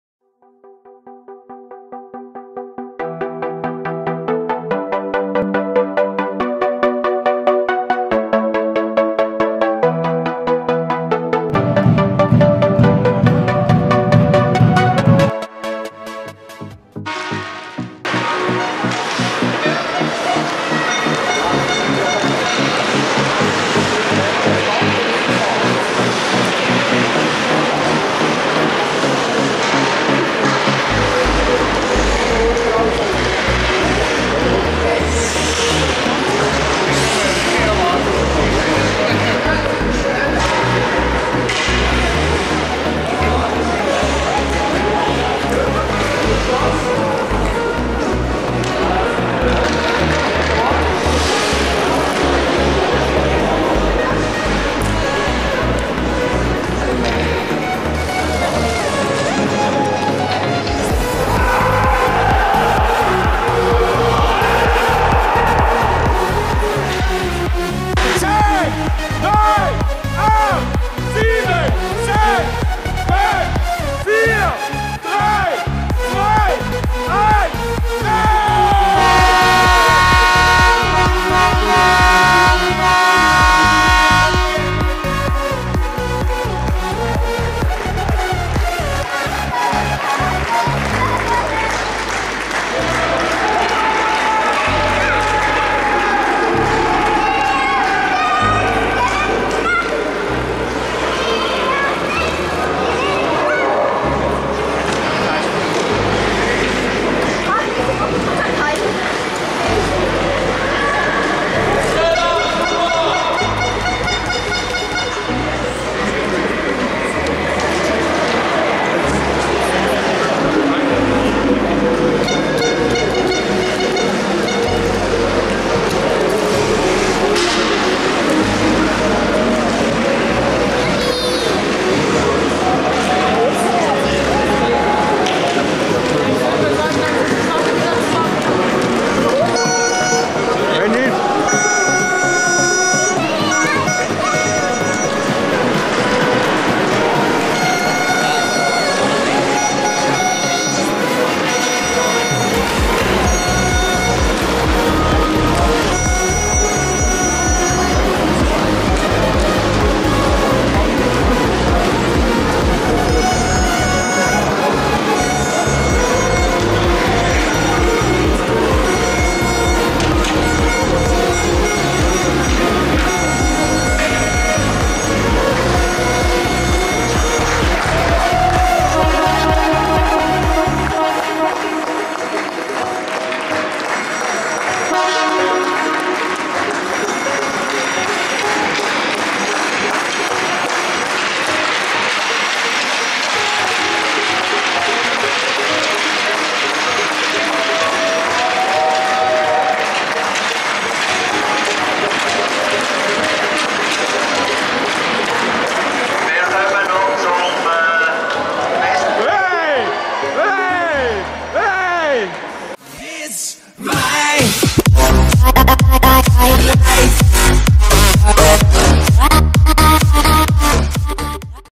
SC Reinach EHC Herrischried nach dem Spiel ist vor dem Spiel
Kleiner einblick ins warm machen vor dem spiel, sowie nach dem spiel verabschiedung, feiern mit den Fans